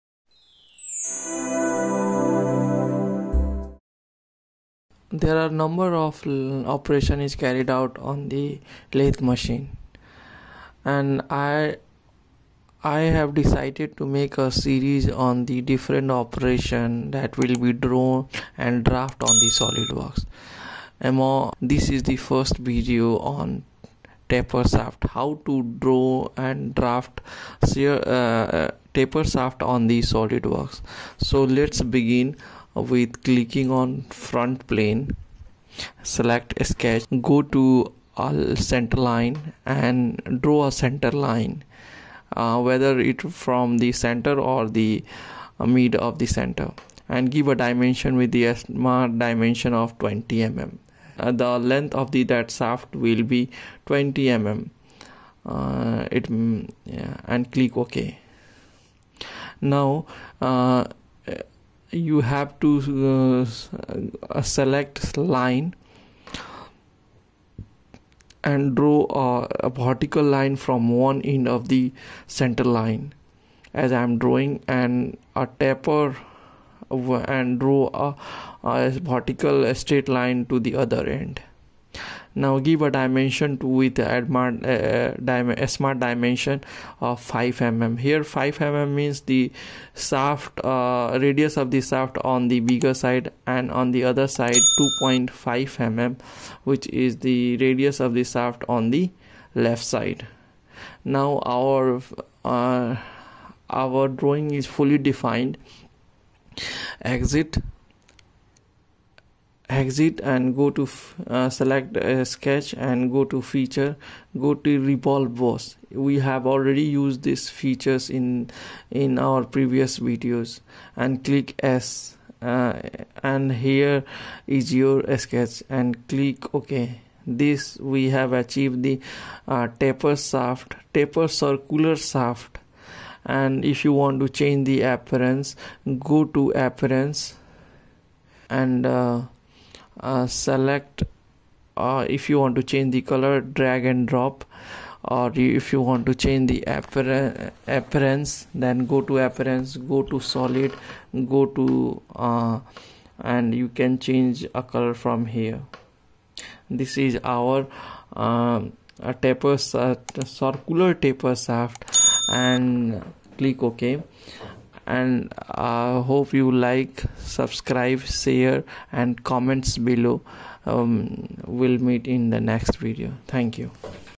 How to draft Tapper Shaft on solid work?
it is full video of drafting tapper shafts . This video contain six of them feature of solid works that is mention below.
extruded Bose/Base
revolved Bose/ase